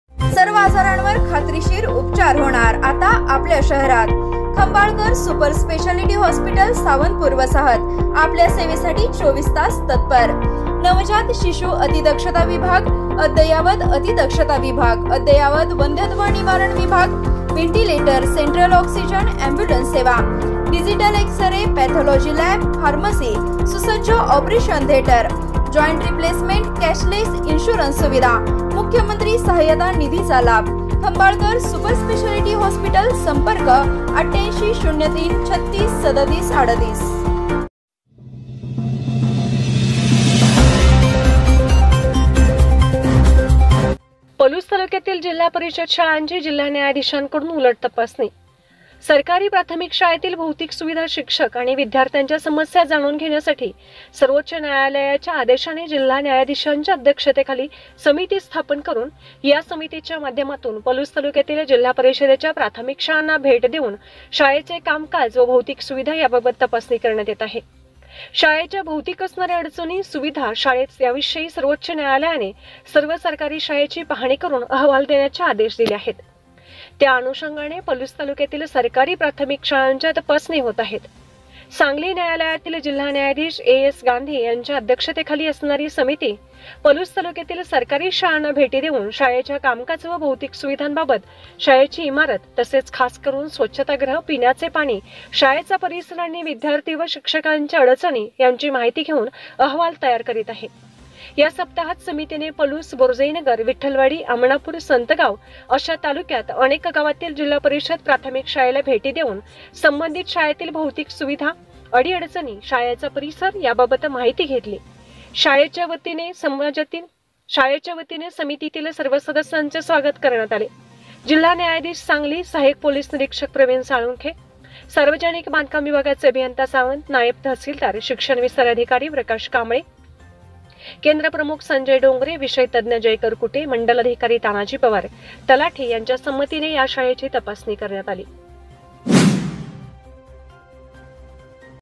ZP SCHOOL | PALUS | JUDGE पलूस तालुक्यातील जि.प.शाळांची जिल्हा न्यायाधीशांकडून उलट तपासणी
पलूस तालुक्यातील जि.प.शाळांची जिल्हा न्यायाधीशांकडून उलट तपासणी